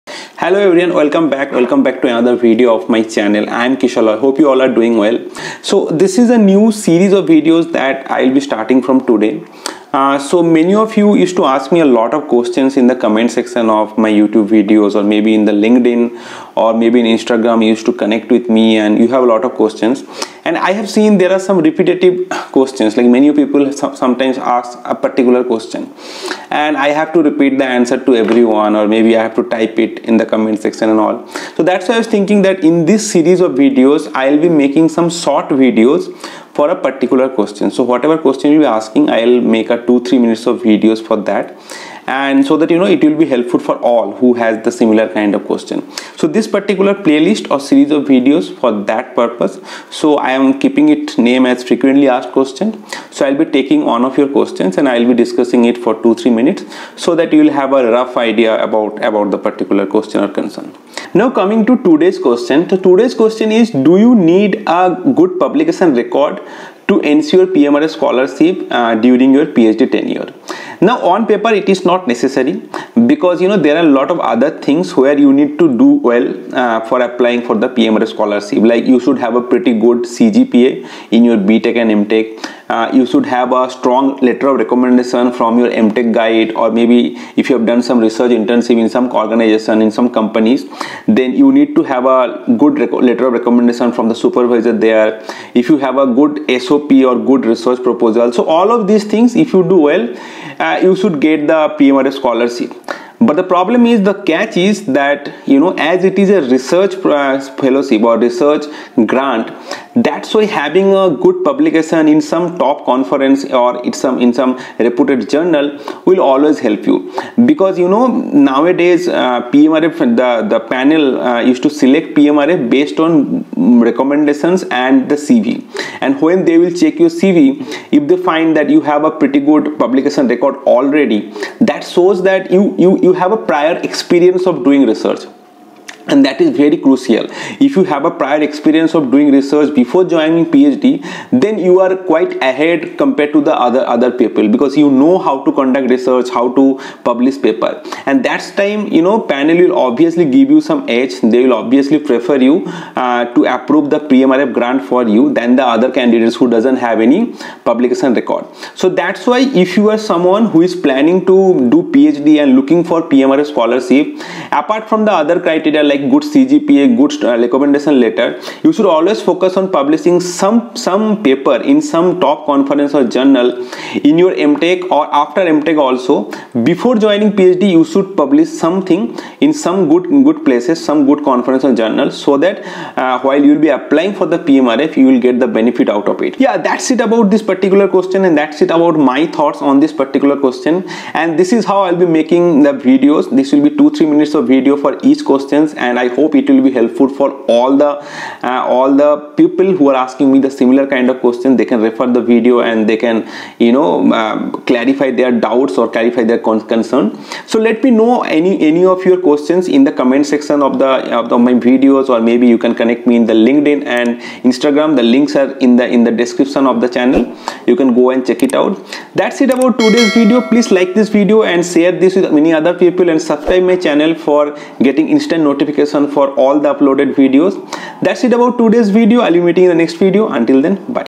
FAQ-01 : Do you need good publication record to ensure PMRF Scholarship?
"Do you need to have good publications for increasing your chance for PMRF? Is it mandetory?"